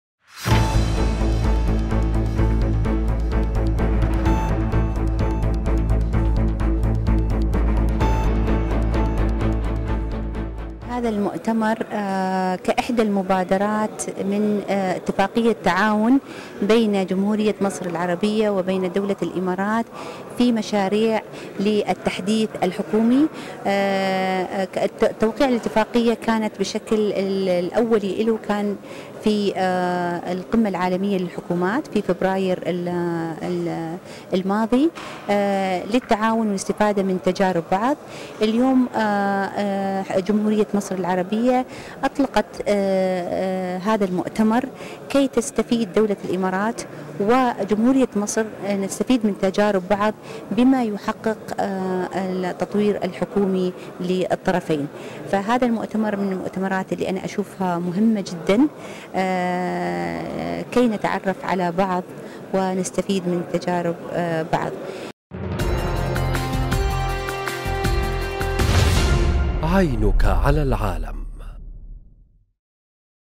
مريم الحمادي: مصر والإمارات تتعاونان في مشاريع التحديث الحكومي
العين الإخبارية | مريم الحمادي: مصر والإمارات تتعاونان في مشاريع التحديث الحكومي
قالت مريم الحمادي، مساعد المدير العام للأداء والتميز الحكومي الإماراتي، إن القاهرة وأبوظبي تتعاونان في مشاريع التحديث الحكومي